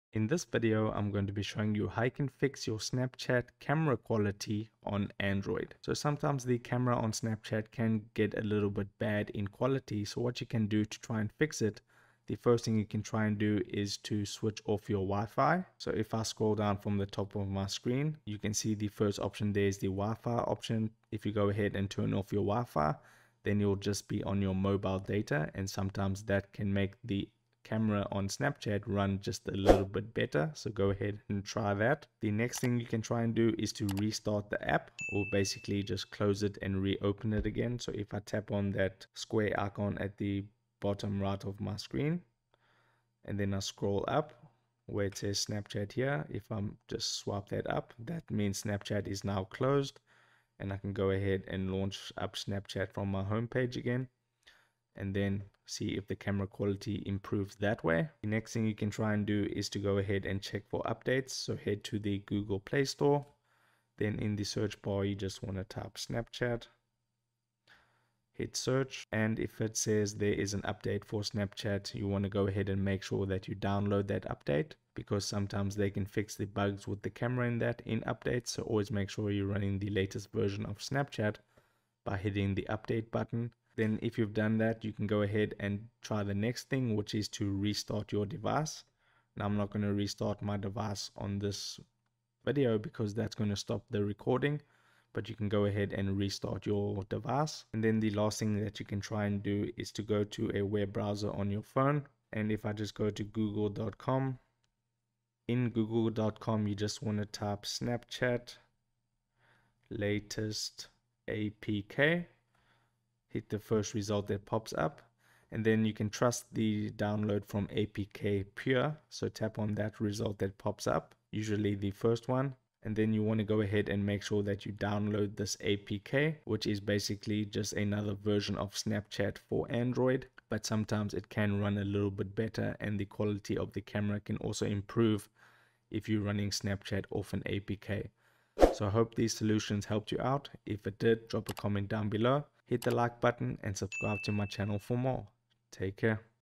How to Fix Snapchat Camera Quality on Android (EASY!)
In this video you will learn how to fix Snapchat camera quality on Android.